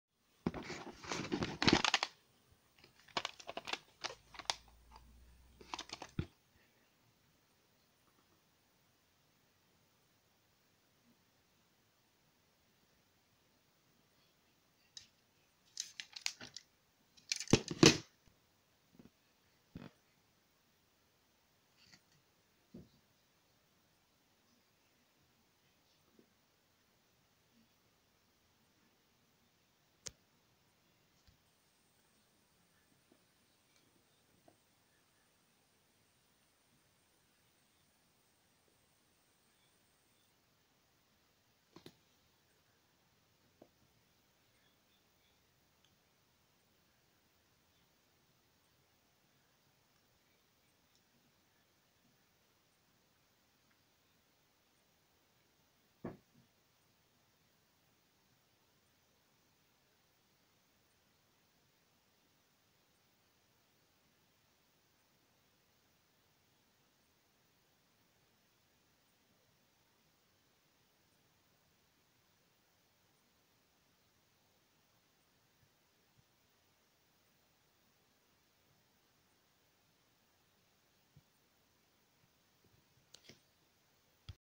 SPOOKY CASTLE Commodore 64 C64 Case Cassette Manual View 31.07.18
SPOOKY CASTLE Commodore 64 C64 Case Cassette Manual View. This version of Spooky Castle was released on the Atlantis label. 31.07.18